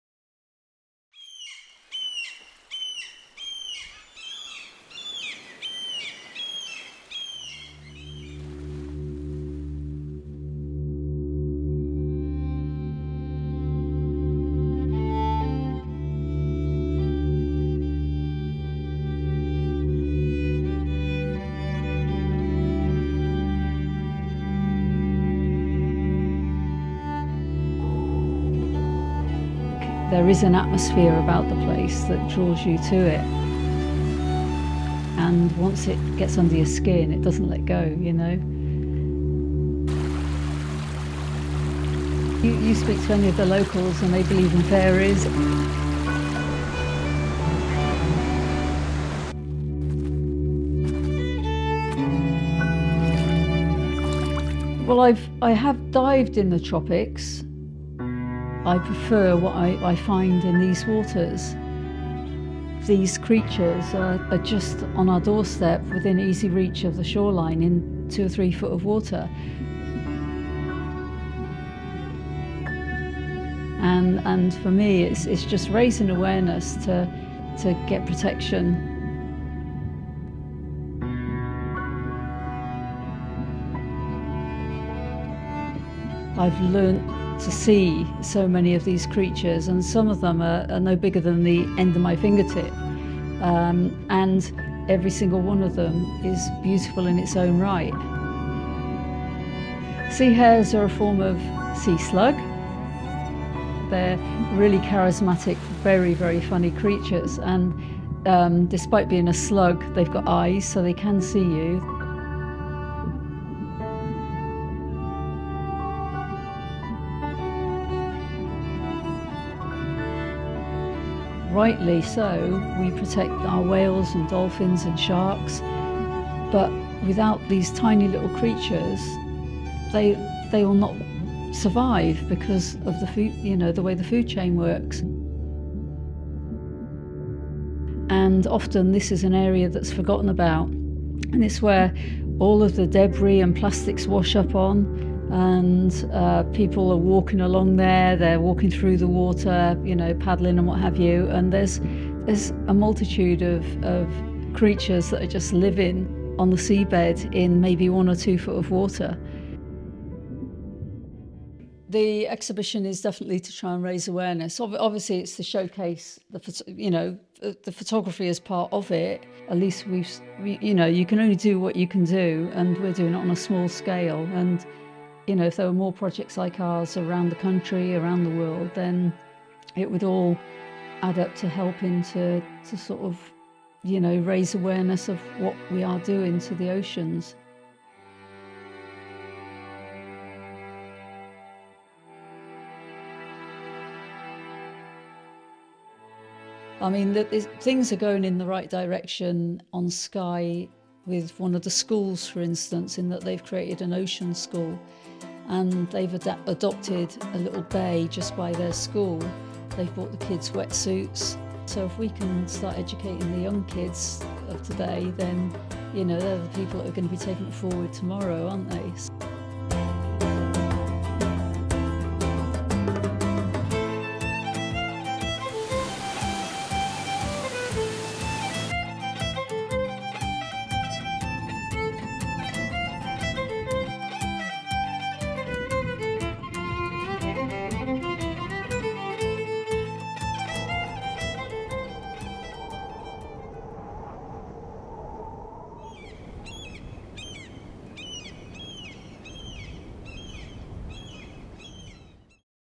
Beneath the Lonesome Skye Zhiyun Documentary shot on iPhone
A documentary shot on iPhone about an underwater photographer on the Isle of Skye, north-west Scotland, raises awareness of the island's fragile marine ecology by focussing on the small creatures essential to the bottom of the food chain.